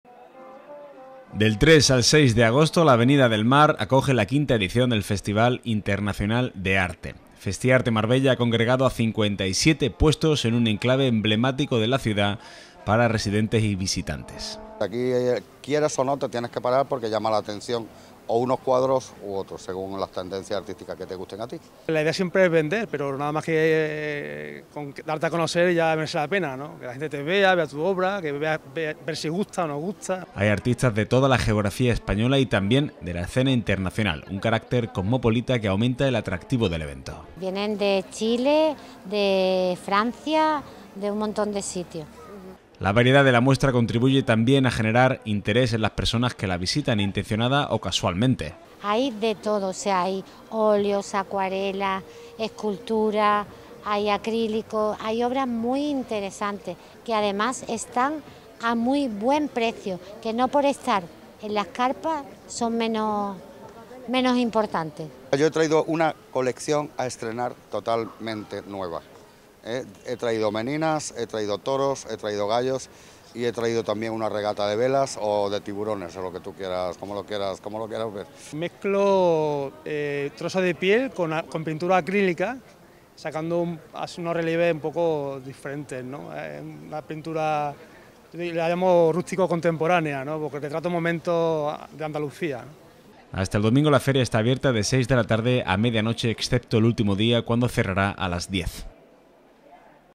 FESTIARTE
Vuelve el Festival Internacional de Arte a Marbella.
Casi 60 puestos con obras muy variadas de artistas llegados de diferentes partes de España y el mundo. El festival internacional de arte cumple cinco ediciones en Marbella. Hasta el domingo, puede visitarlo en la Avenida del Mar, de seis de la tarde a medianoche.